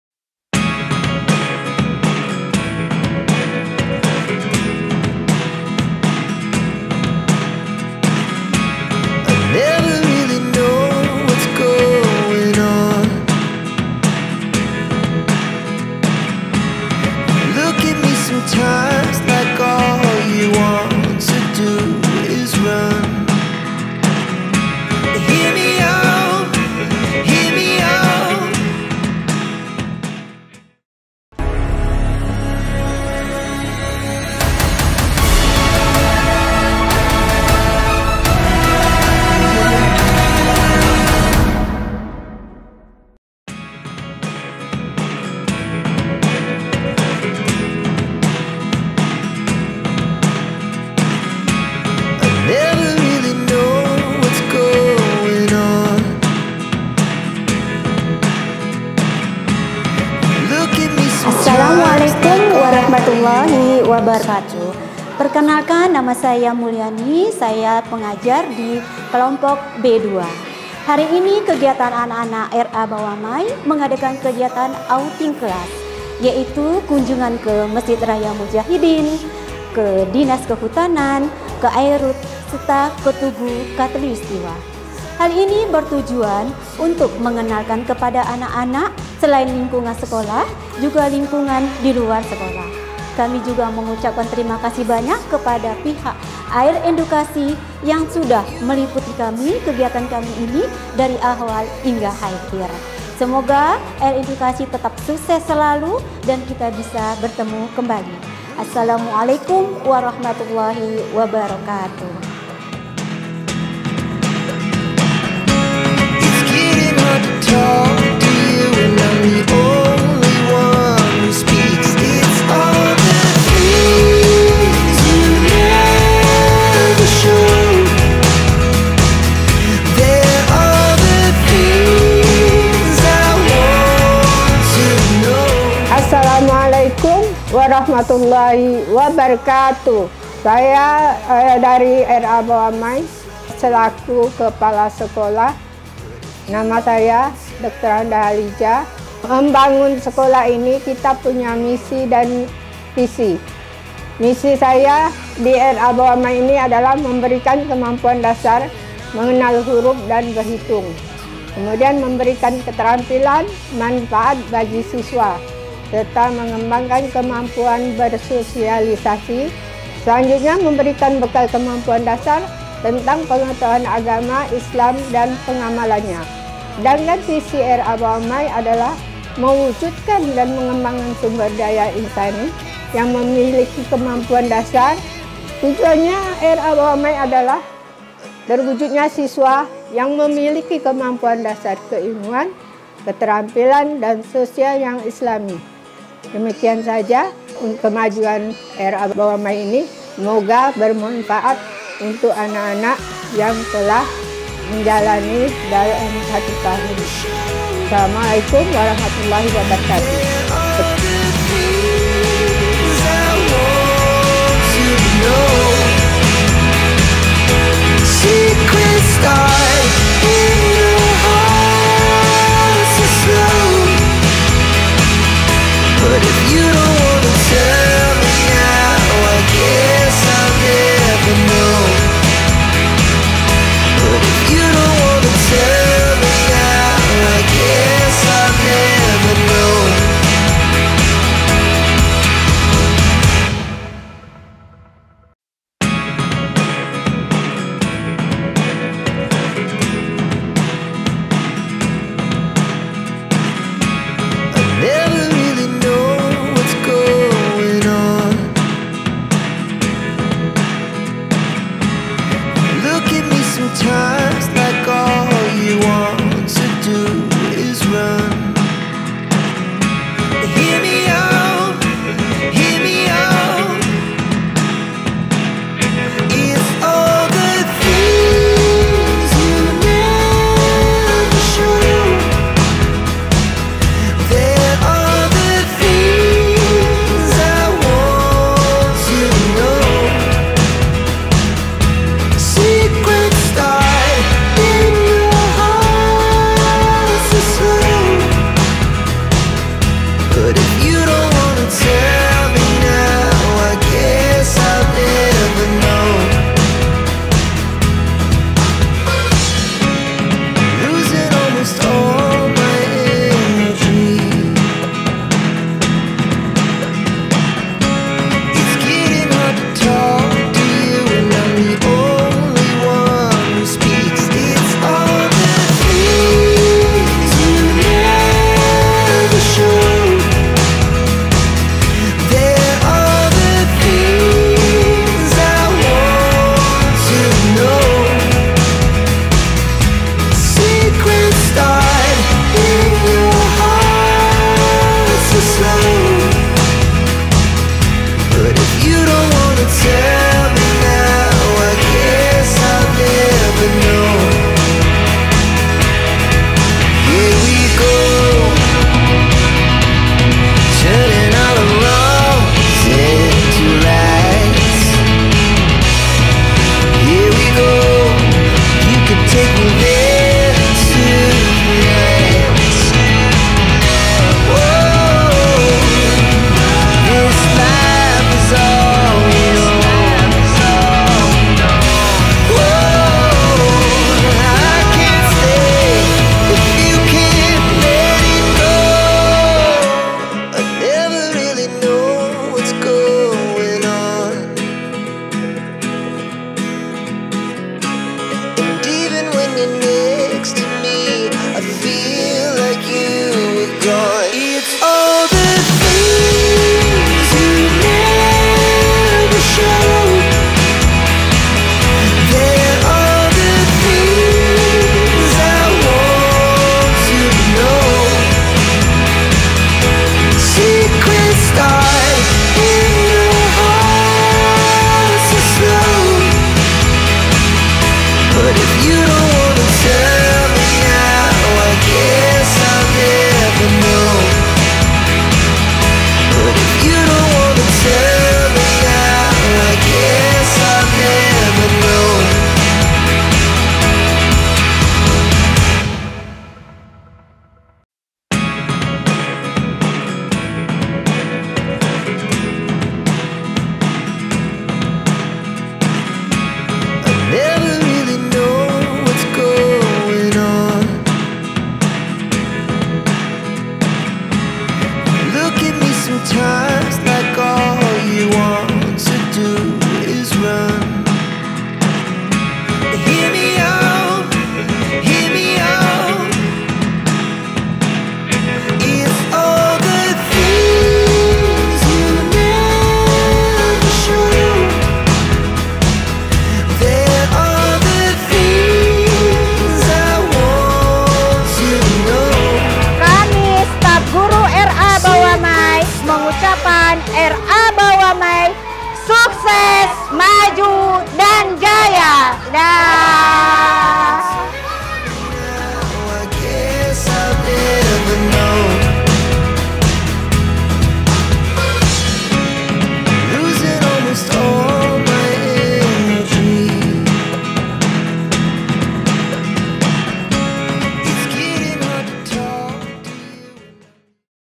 Film Dokumenter : Petualangan Cerdas Di Taman Belajar /TK BAWAMAI /Pontianak Kota
Sebuah Karya Film Dokumenter RA Bawamai Kota Pontianak

Tema             : Edukasi TK BAWAMAI
Kategori        : Dokumenter
Production   : AIR Edukasi

↓ Thanks To  ↓

TK BAWAMAI